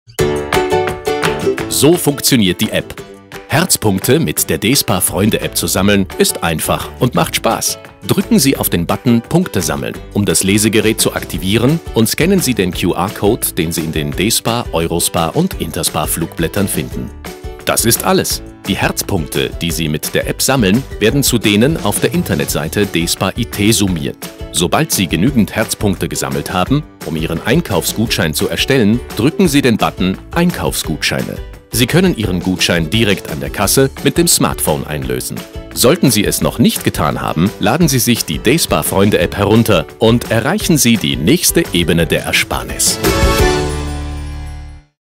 Die Despar Freunde-App
Finden Sie heraus, wie Sie mit Ihrem Handy Herzpunkte sammeln können - mit der neuen Despar Freunde-App.